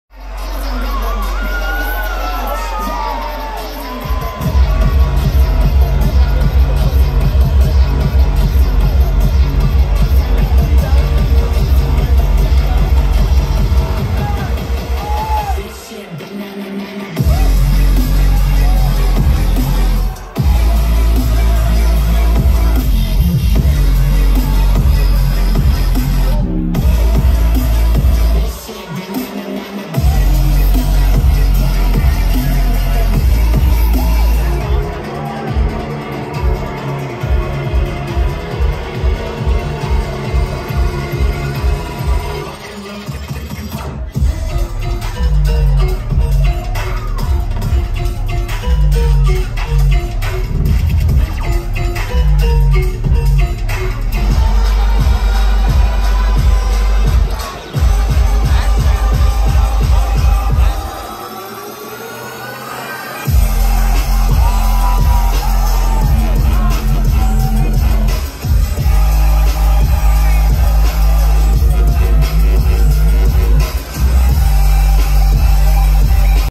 Wolf-E-Wolf @ Fox Theatre 2022
Recap Of Wolf-E-Wolf @ Fox Theatre, Boulder, CO
Date of Show: 5/28/2022

Follow @redsedm on Instagram, Twitter, & Tik Tok Plz

Lineup:
Jantsen + B5B with Tvboo, Space Wizard, Wolf-E-Wolf, & Komuz
Wolf-E-Wolf
IAM_Jacko
Krushendo

Comment for IDs